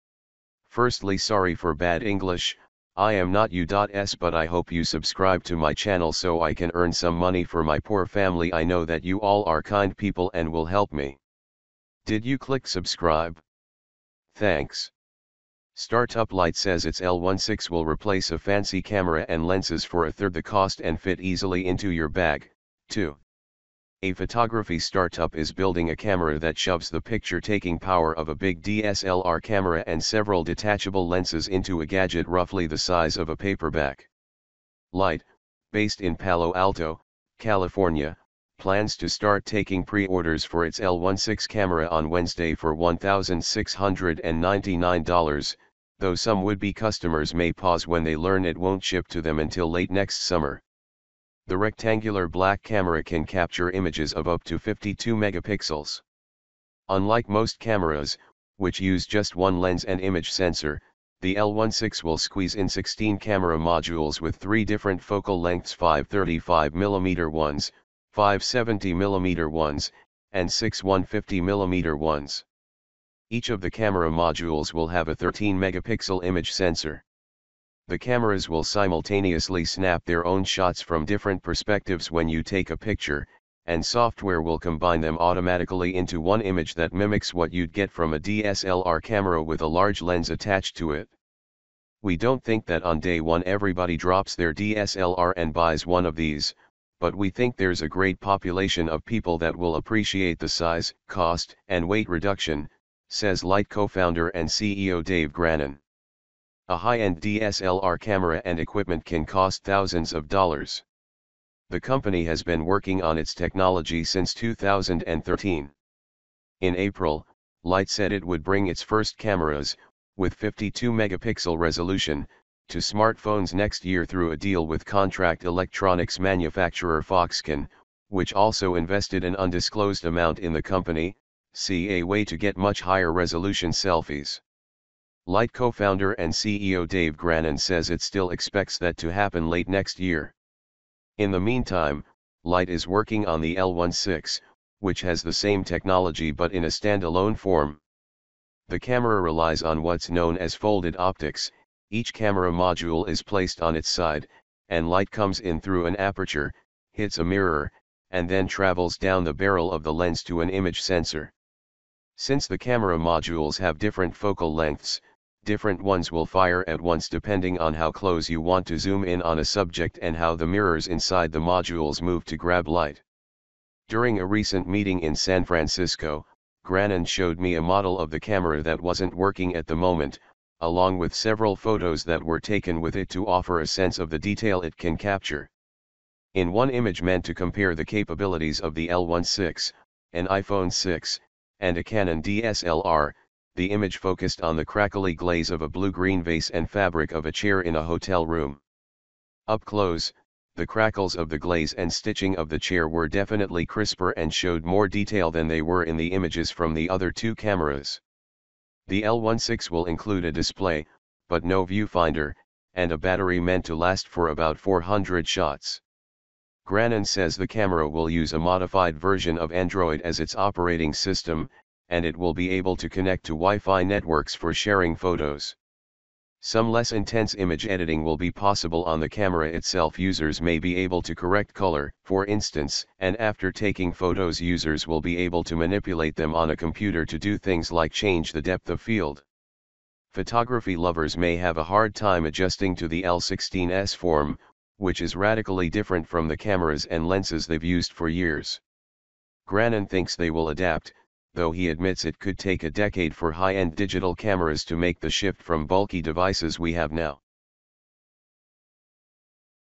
A High End Camera in a Small Package
Startup Light says its L16 will replace a fancy camera and lenses for a third the cost and fit easily into your bag, too.
A photography startup is building a camera that shoves the picture-taking power of a big DSLR camera and several detachable lenses into a gadget roughly the size of a paperback.

Light, based in Palo Alto, California, plans to start taking preorders for its L16 camera on Wednesday for $1,699, though some would-be customers may pause when they learn it won’t ship to them until late next summer.

The rectangular black camera can capture images of up to 52 megapixels. Unlike most cameras, which use just one lens and image sensor, the L16 will squeeze in 16 camera modules with three different focal lengths—five 35-millimeter ones, five 70-millimeter ones, and six 150-millimeter ones. Each of the camera modules will have a 13-megapixel image sensor. The cameras will simultaneously snap their own shots from different perspectives when you take a picture, and software will combine them automatically into one image that mimics what you’d get from a DSLR camera with a large lens attached to it.

“We don’t think that on day one everybody drops their DSLR and buys one of these, but we think there’s a great population of people that will appreciate the size, cost, and weight reduction,” says Light cofounder and CEO Dave Grannan. A high-end DSLR camera and equipment can cost thousands of dollars.
The company has been working on its technology since 2013. In April, Light said it would bring its first cameras, with 52-megapixel resolution, to smartphones next year through a deal with contract electronics manufacturer Foxconn, which also invested an undisclosed amount in the company (see “A Way to Get Much Higher-Resolution Selfies”). Light cofounder and CEO Dave Grannan says it still expects that to happen late next year.

In the meantime, Light is working on the L16, which has the same technology but in a stand-alone form. The camera relies on what’s known as folded optics: each camera module is placed on its side, and light comes in through an aperture, hits a mirror, and then travels down the barrel of the lens to an image sensor. Since the camera modules have different focal lengths, different ones will fire at once depending on how close you want to zoom in on a subject and how the mirrors inside the modules move to grab light.

During a recent meeting in San Francisco, Grannan showed me a model of the camera that wasn’t working at the moment, along with several photos that were taken with it to offer a sense of the detail it can capture.

In one image meant to compare the capabilities of the L16, an iPhone 6, and a Canon DSLR, the image focused on the crackly glaze of a blue-green vase and fabric of a chair in a hotel room. Up close, the crackles of the glaze and stitching of the chair were definitely crisper and showed more detail than they were in the images from the other two cameras.

The L16 will include a display (but no viewfinder) and a battery meant to last for about 400 shots. Grannan says the camera will use a modified version of Android as its operating system, and it will be able to connect to Wi-Fi networks for sharing photos. Some less-intense image editing will be possible on the camera itself—users may be able to correct color, for instance—and after taking photos users will be able to manipulate them on a computer to do things like change the depth of field.

Photography lovers may have a hard time adjusting to the L16’s form, which is radically different from the cameras and lenses they’ve used for years. Grannan thinks they will adapt, though he admits it could take a decade for high-end digital cameras to make the shift from bulky devices we have now.